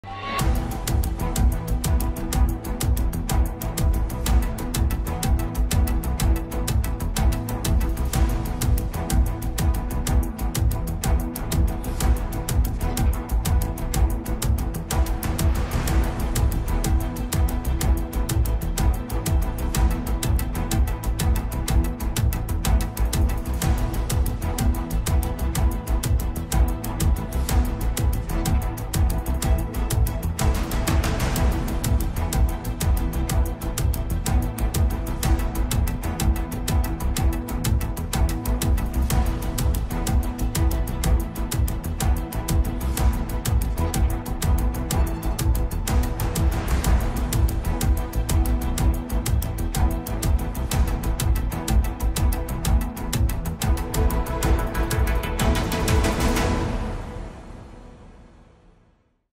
Australian beaches closed as sharks feed on whale carcass | WION Shorts
A whale carcass off the coast of Rottnest Island in Western Australia sparked a shark feeding frenzy
there were around 30 sharks feeding on the whale, including tiger sharks and bronze whalers.